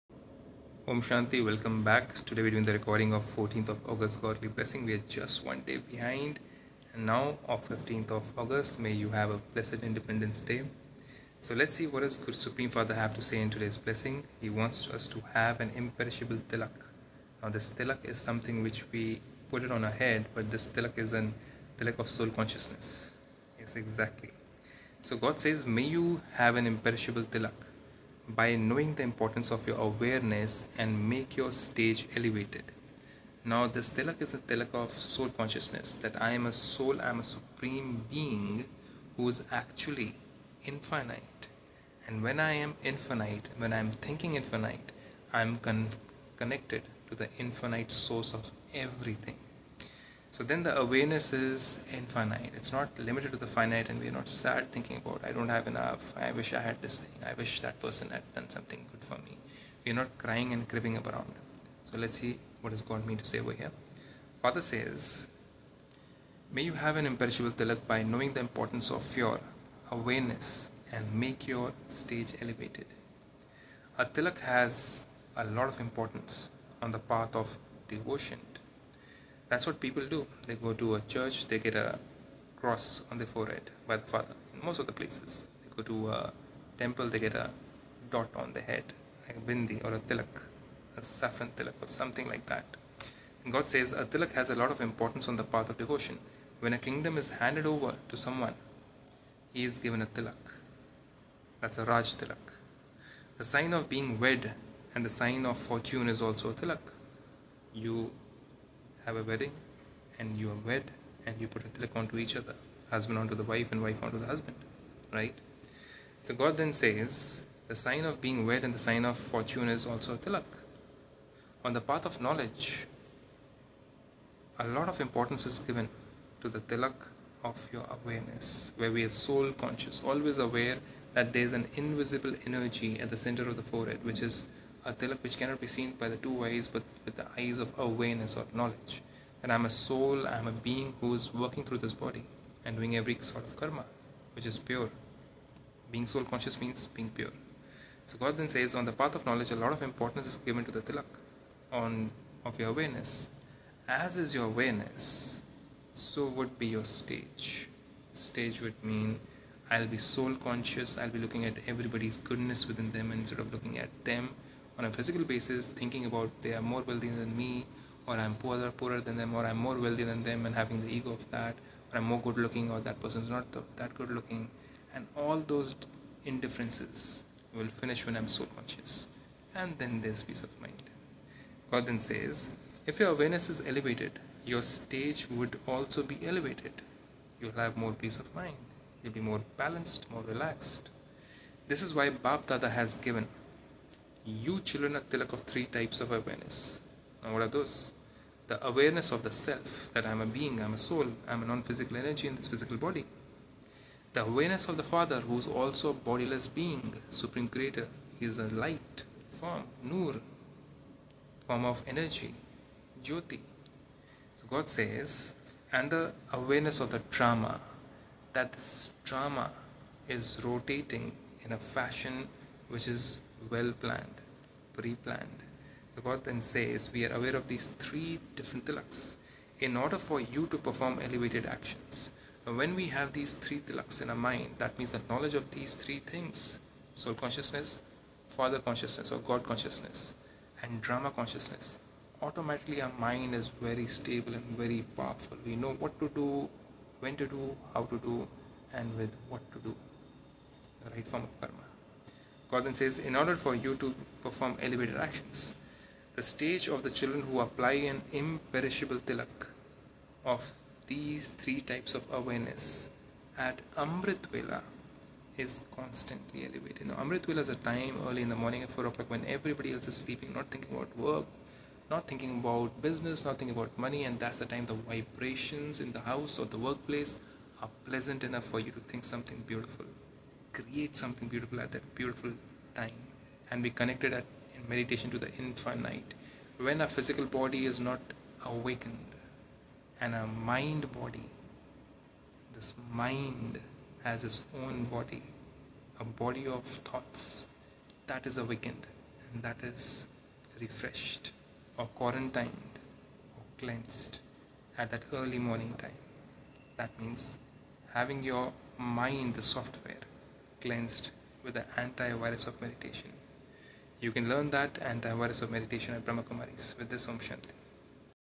Have an imperishable tilak by knowing the importance of your awareness & make your stage elevated.
Day 195 - May you have an imperishable tilak by knowing the importance of your awareness and make your stage elevated.

Om Shanti .